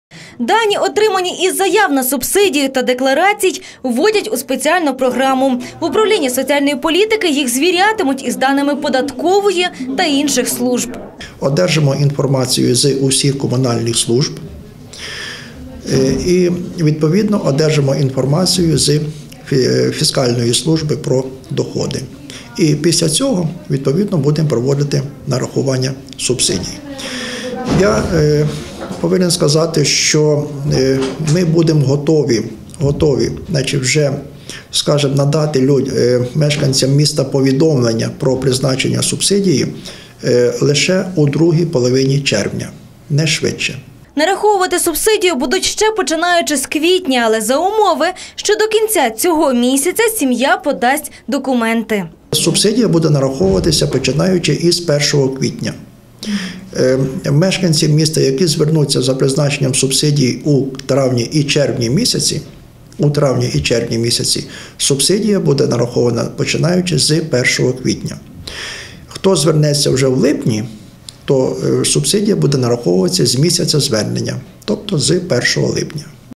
Дані в заявах та деклараціях, подані на субсидії, перевіряють
Усі дані в заявах та деклараціях, подані на субсидії, перевіряють. Таке завдання стоїть перед працівниками соціального управління. ПРодовжується і прийом документів. У міськраді їх рекомендують приносити особисто. Документи приймають в усіх ЖЕКах міста, а також в управлінні соціальної політики. Тим, хто подасть заяву і декларацію до кінці червня, субсидію нарахують ще з 1 квітня.